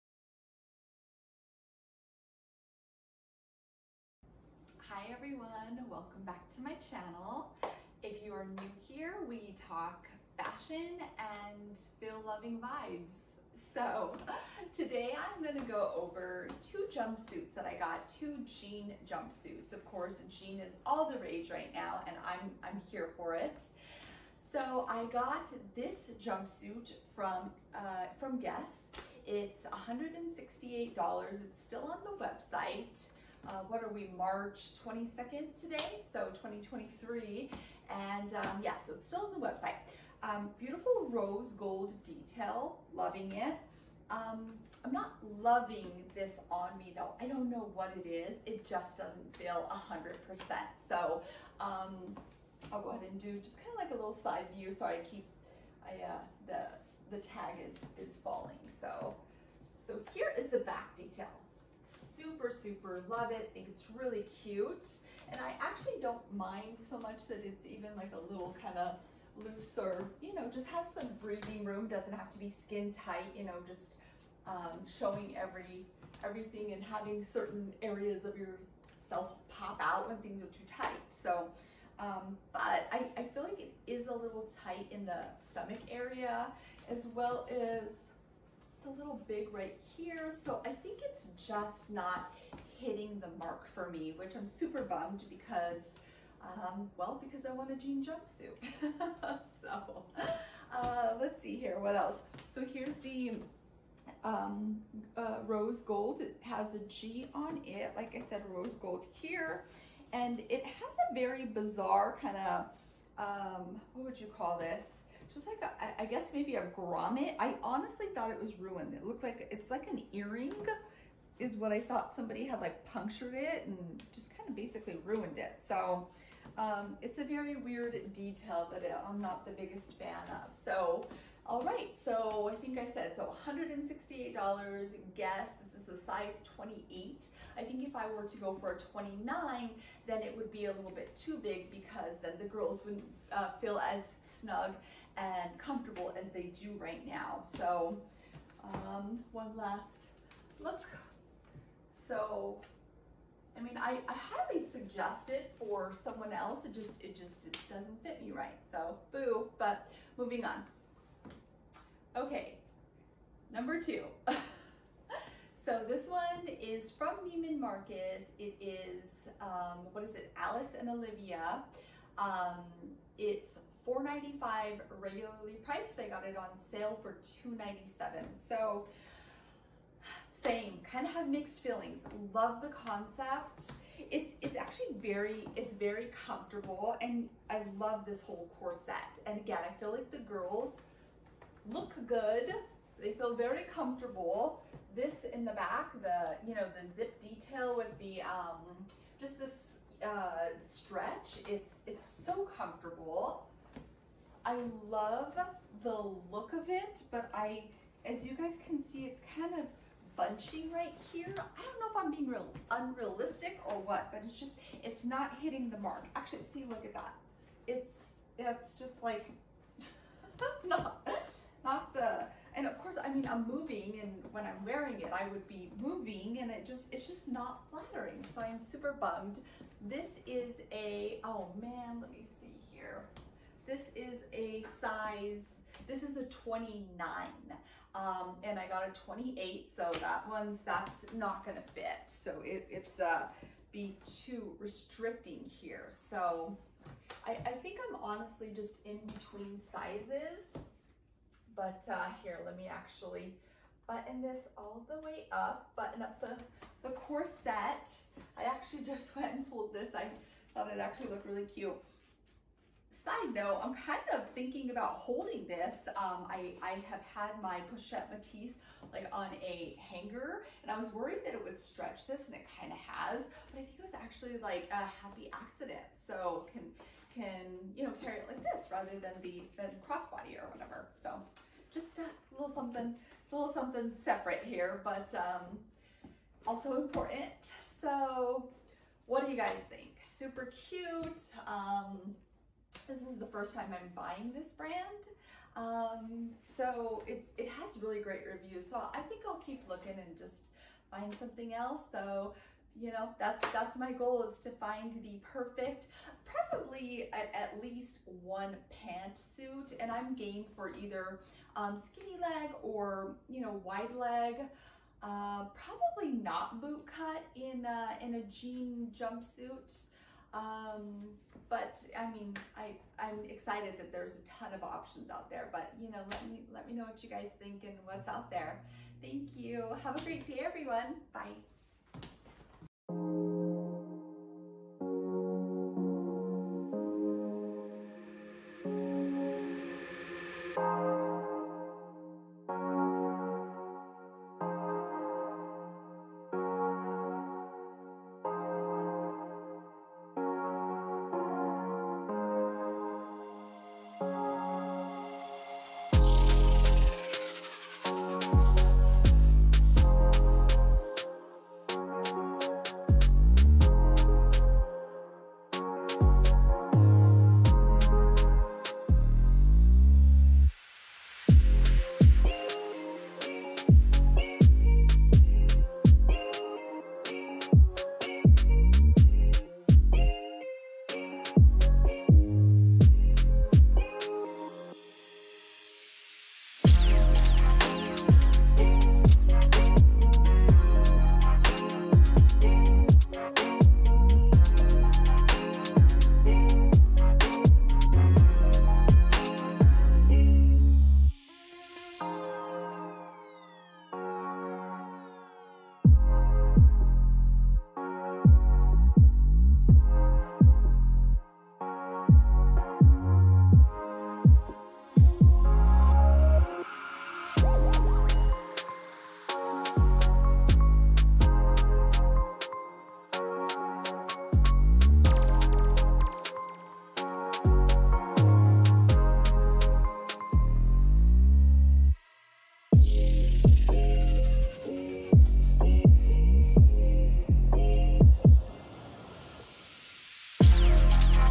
Guess Alice + Olivia Jean Jump Suites Try On @GUESS @aliceandolivia
Guess Alice + Olivia Jean Jump Suites Try On. I will share with you my thoughts on each jumpsuit, size, style and fit. Enjoy!